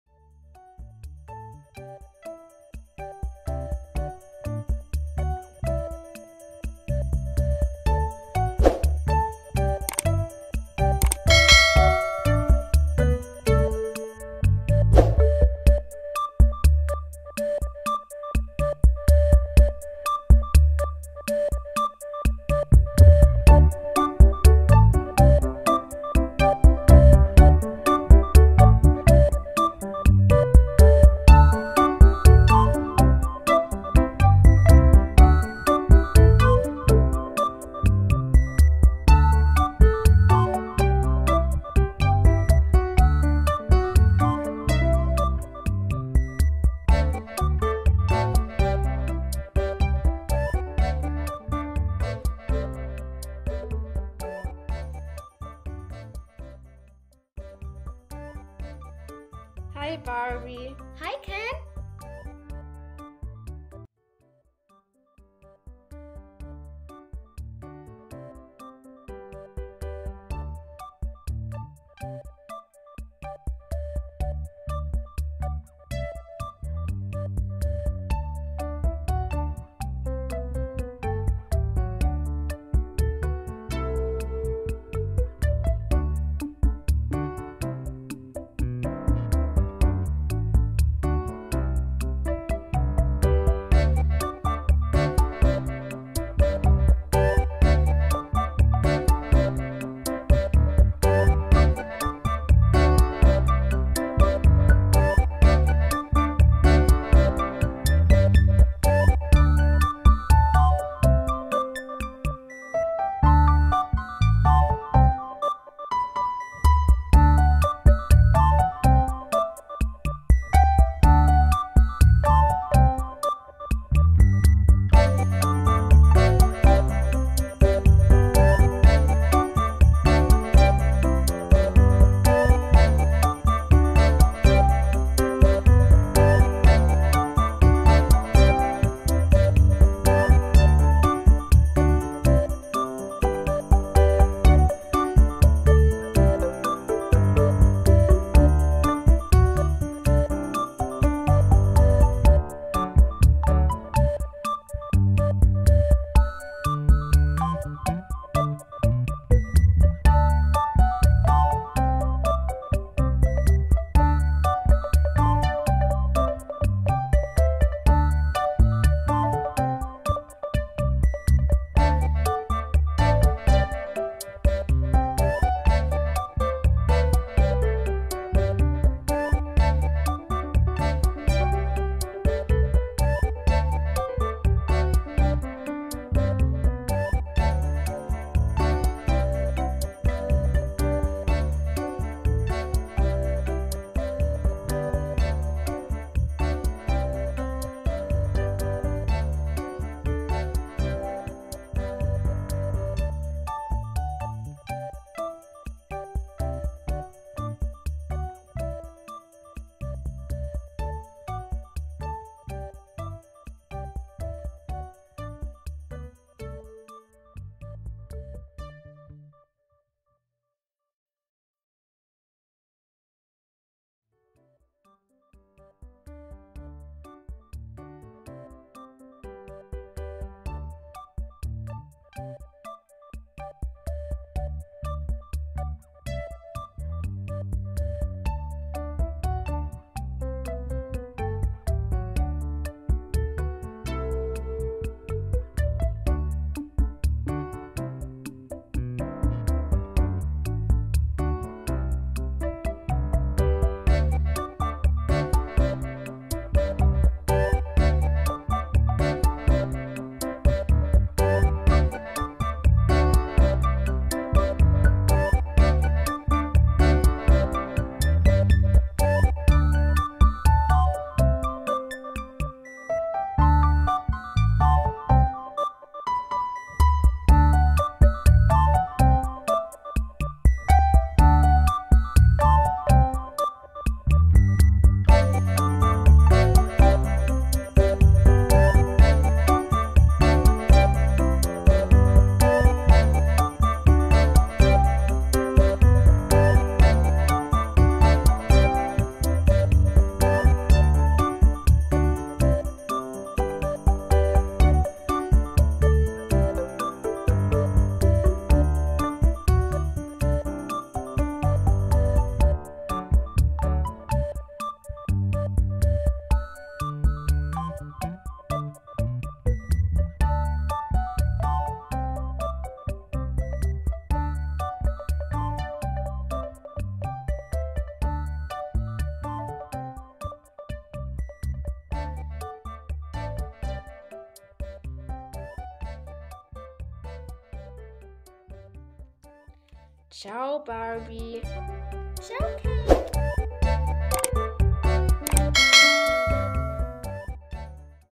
MUÑECA DE PAPEL para VESTIR. La Ropa de Barbie en la película
Barbie y Ken PaperDoll DressUp. playing with QUIETBOOK. La Ropa de Barbie en la película 2023